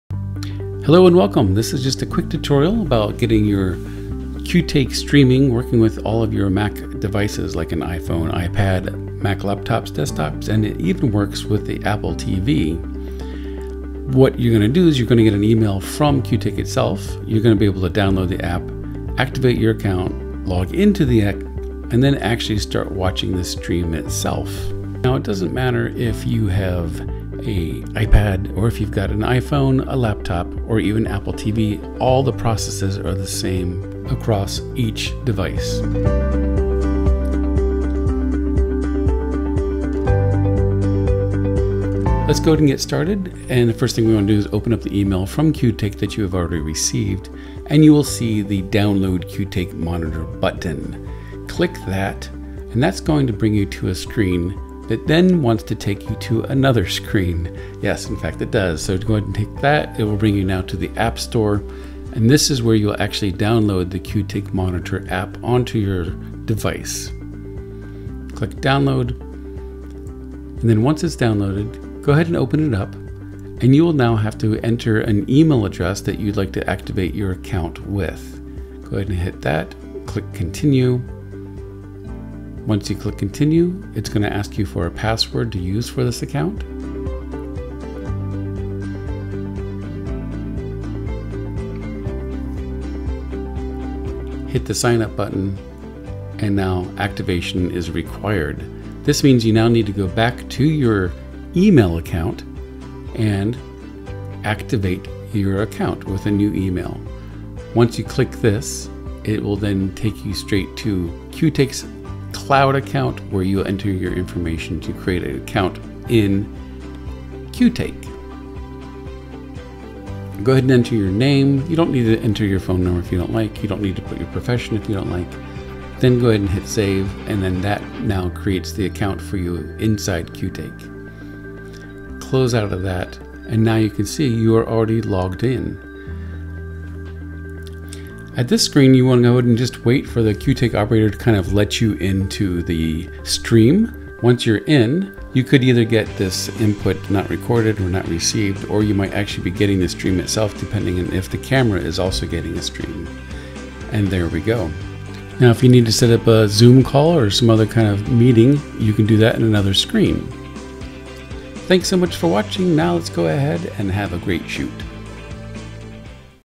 Qtake Stream Tutorial - Lost Dog Video
This is an in depth tutorial on the process of downloading the Qtake app on your Apple devices and creating your Qtake account in order to be able to see your on set remote feed.